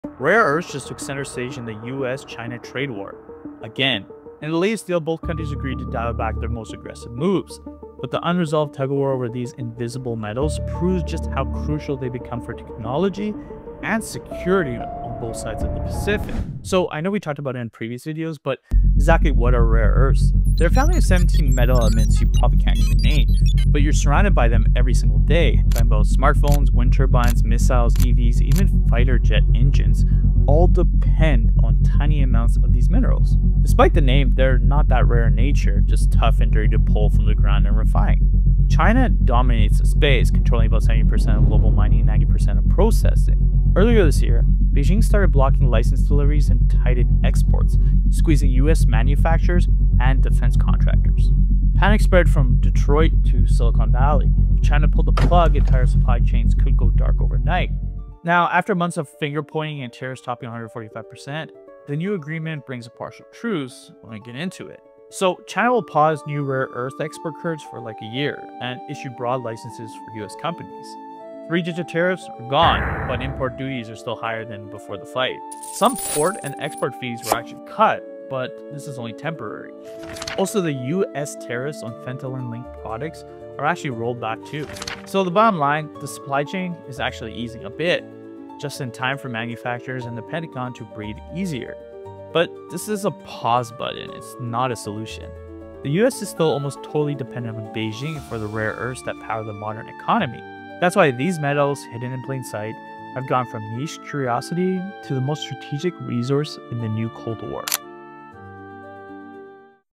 Rare Earths: The Resource China Still Controls
Rare earth metals just returned to the center of the US–China power struggle. They’re in EVs, missiles, smartphones, wind turbines—everything modern economies run on.

A temporary truce is in place, but the dependency remains.